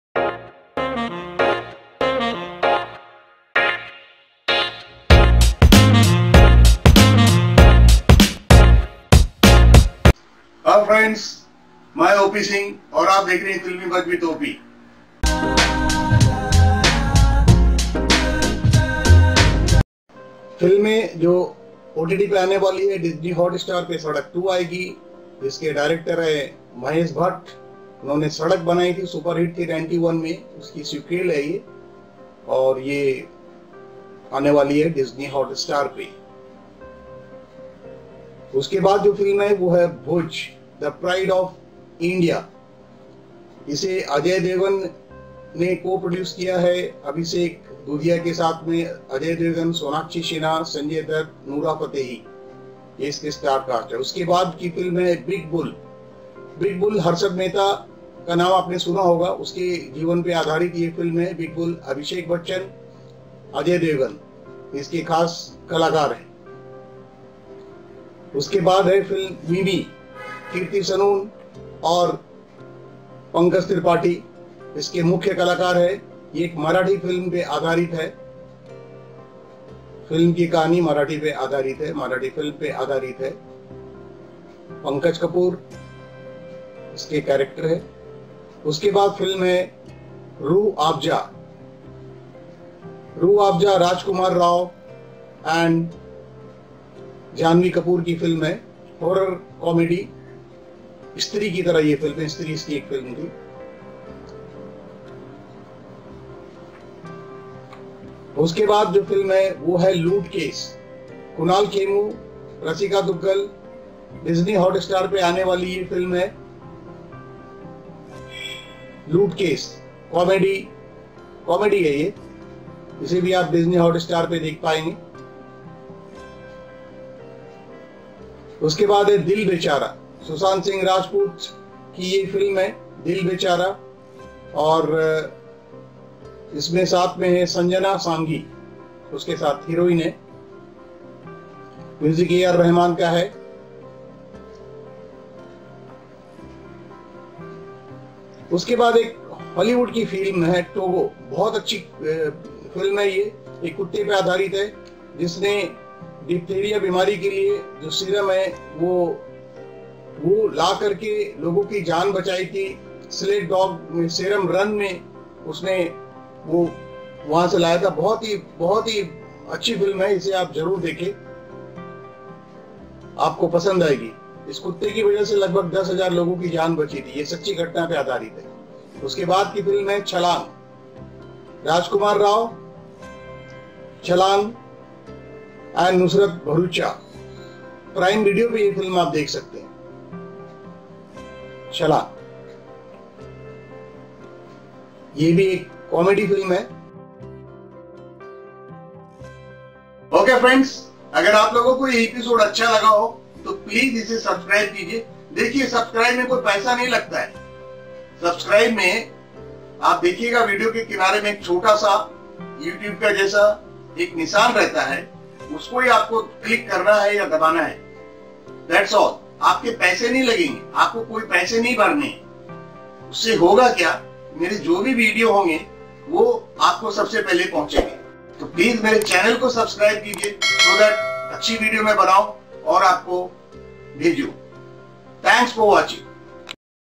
NEW /GOOD FILMS ON OTT PLATFORMS/DISNEY+HOTSTAR/PRIME VIDEO - HINDI
New /Good films on OTT platforms, What are the films you can watch in June 2020 and some of them are still to be a stream, all good films, specially TOGO on Disney + Hotstar.
All images /videos have been used for creative examples/good use.